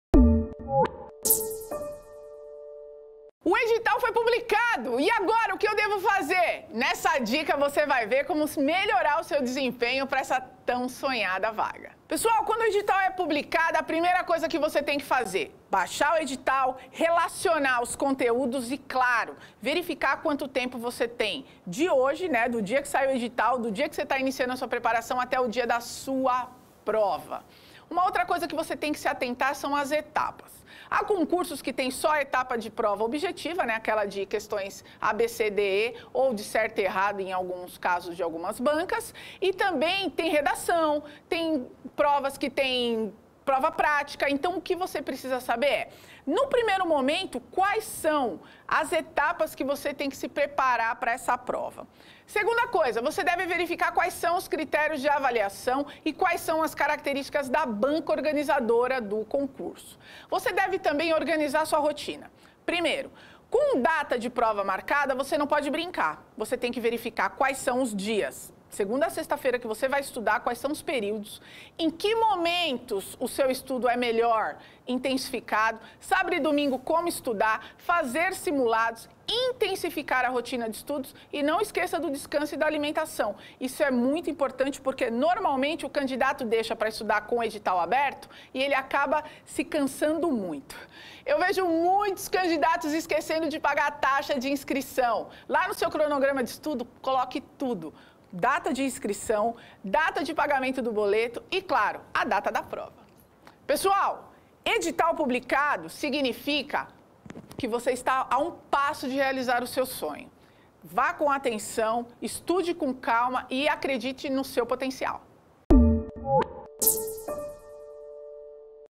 DICA - O que fazer quando o Edital é PUBLICADO?
Confira a dica da Viviane Rocha, professora de Planejamento Estratégico de Estudos e Mestre em Psicologia Educacional e Processo de Aprendizagem na idade adulta